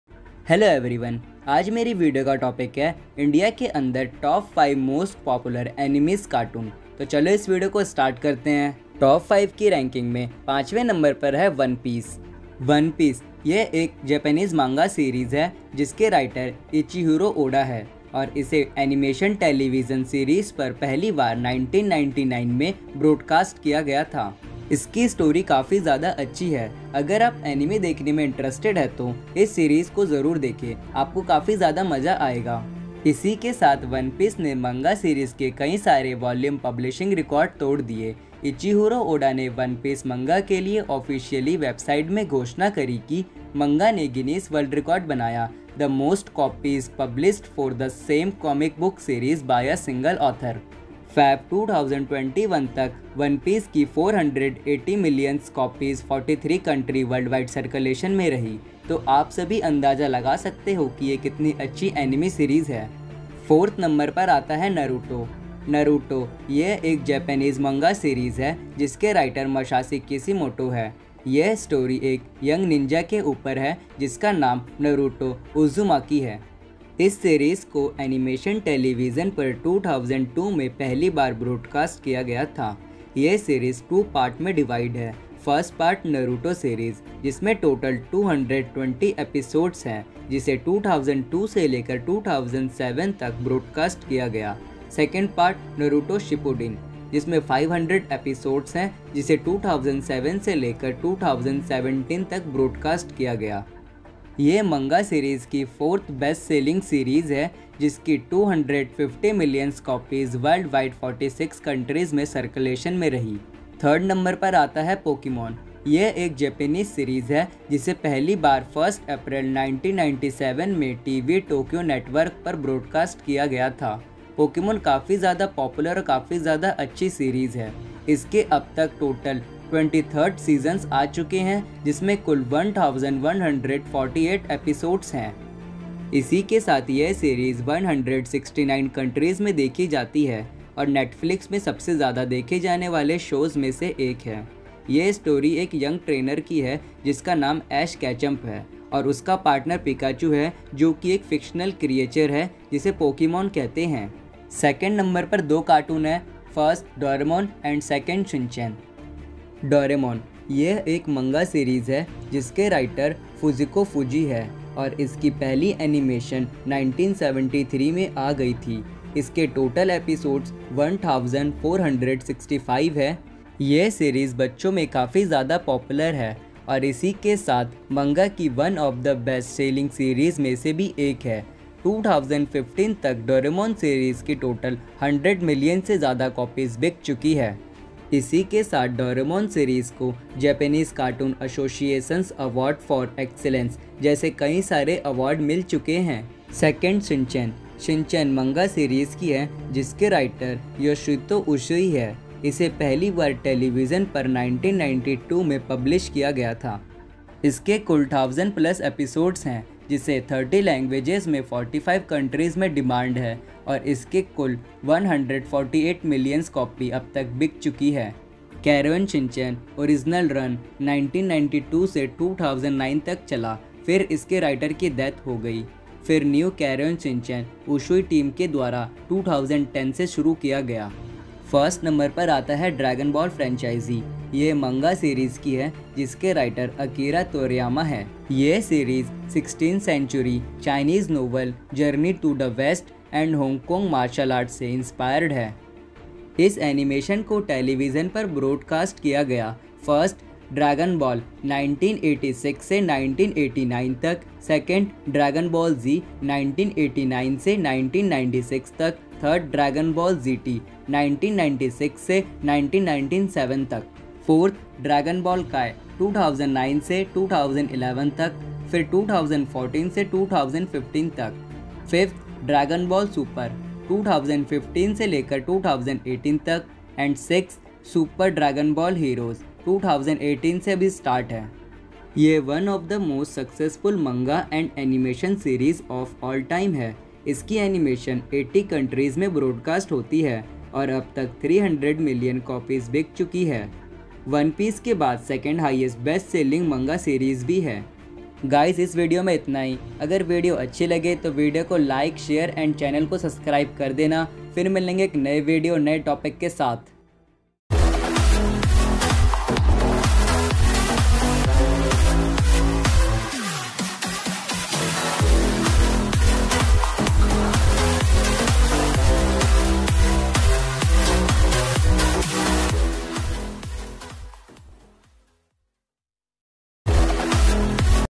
Top 5 Animes In India || Full Video In Hindi.
Is Video Jo Jankari De Jaa Rahai Hai Wo Sabhi Internet Main mile Imbi Rating Ke According Hai.

I Hope Aap Sabhi Ko ye Video Achi lage.